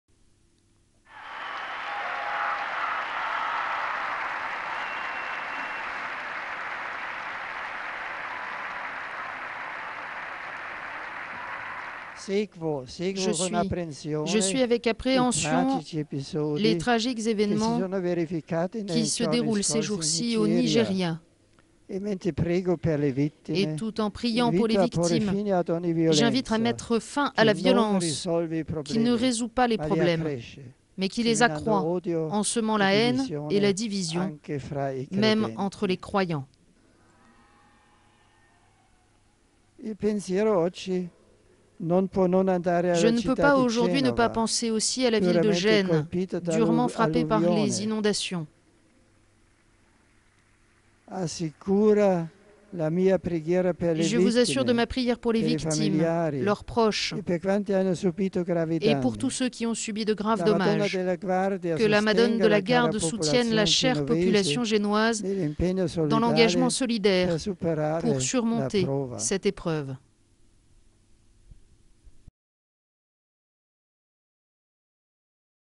Le Pape prie pour les victimes au Nigeria et à Gênes
Je suis avec appréhension les tragiques événements qui se déroulent ces jours ci au Nigéria et, tout en priant pour les victimes, j'invite à mettre fin à la violence, qui ne résoud pas les problèmes mais les accroît en semant la haine et la division même entre les croyants. (...)
Je ne peux pas aujourd'hui ne pas penser aussi à la ville de Gênes, durement frappée par les inondations. Je vous assure de ma prière pour les victimes, leurs proches, et tous ceux qui ont subi de graves dommages. Que la Madone de la Garde soutienne la chère population génoise dans l'engament solidaire pour surmonter cette épreuve.